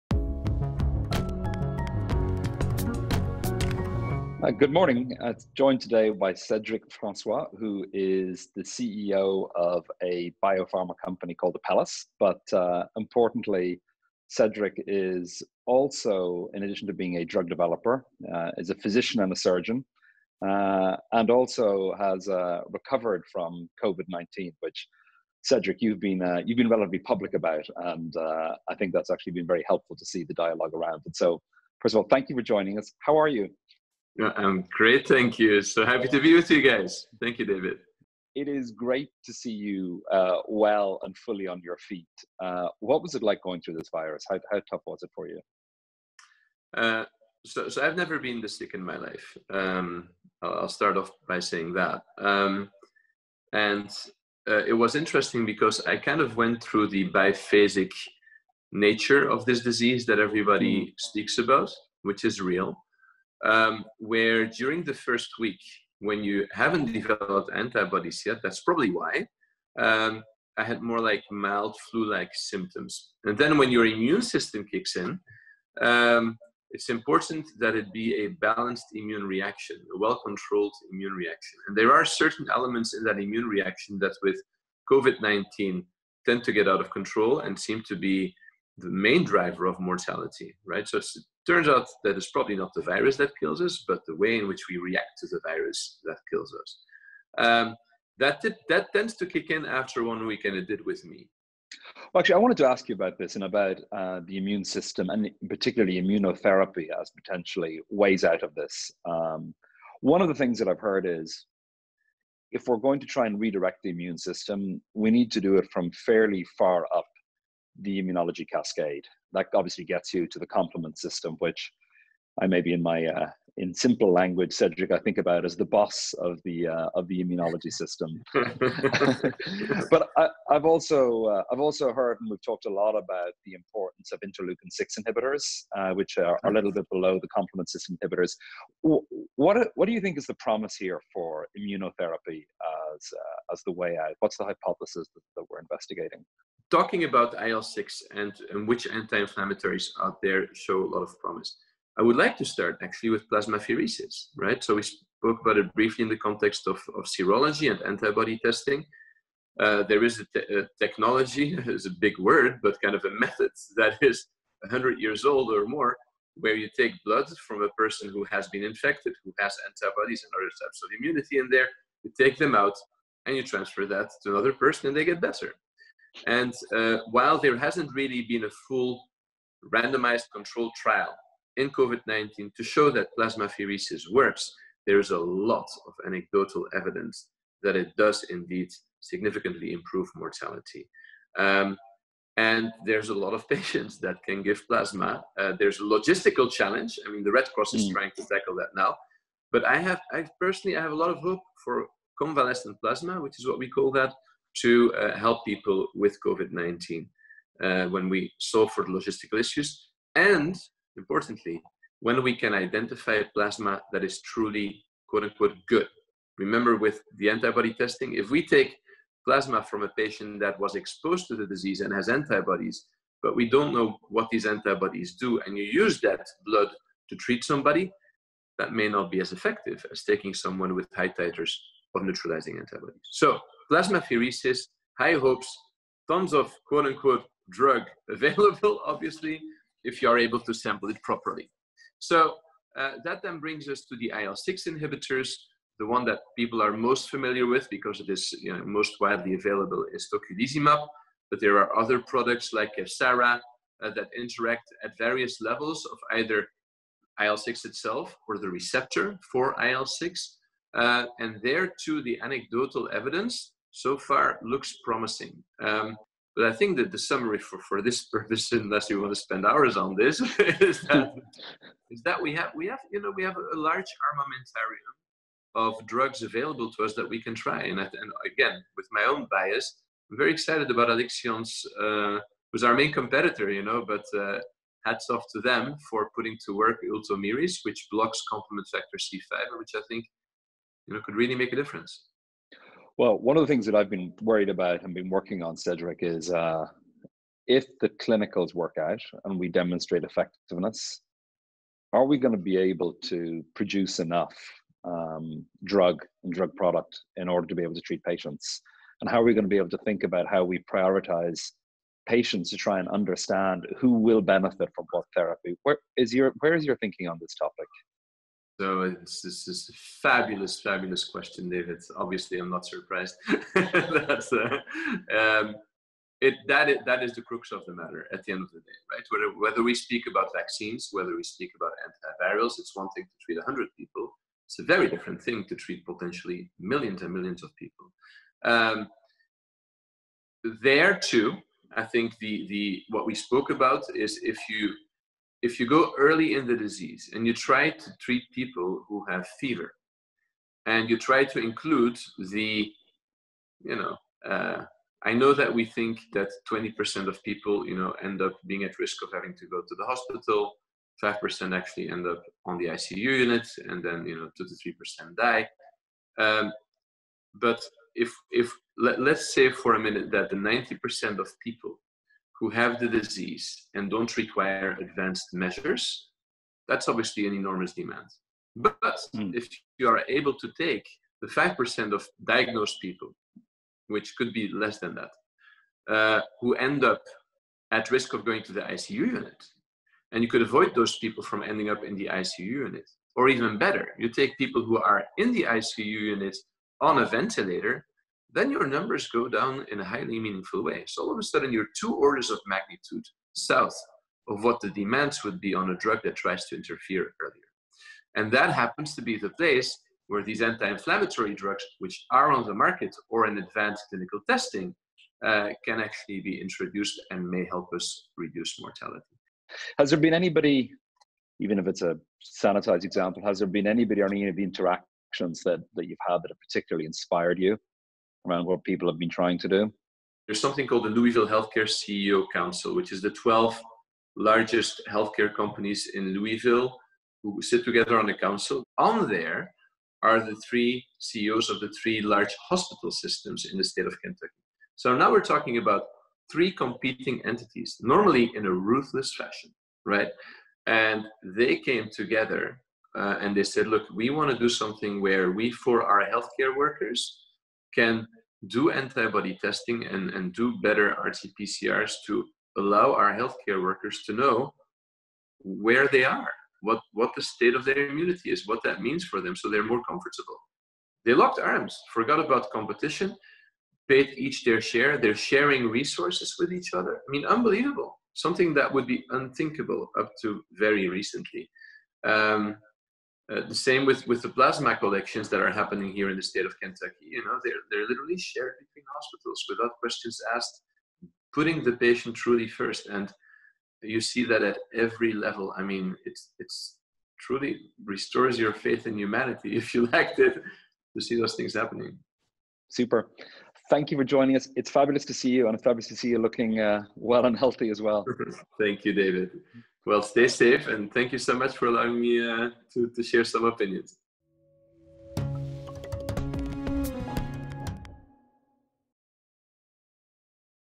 Treating COVID-19: A pharmaceutical executive’s view
Cedric Francois, the CEO of pharmaceutical company Apellis, discusses possible approaches to treating COVID-19 patients.